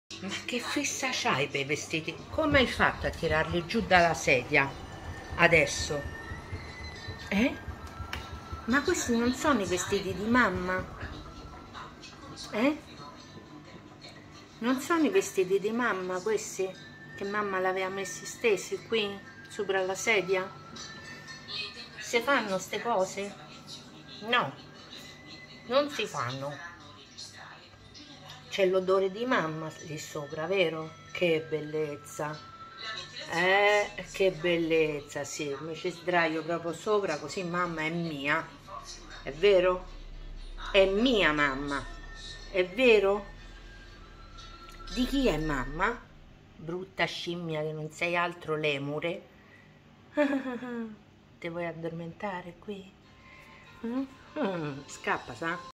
Mila felice ruba i vestiti di mamma e ci scangura sopra
gattini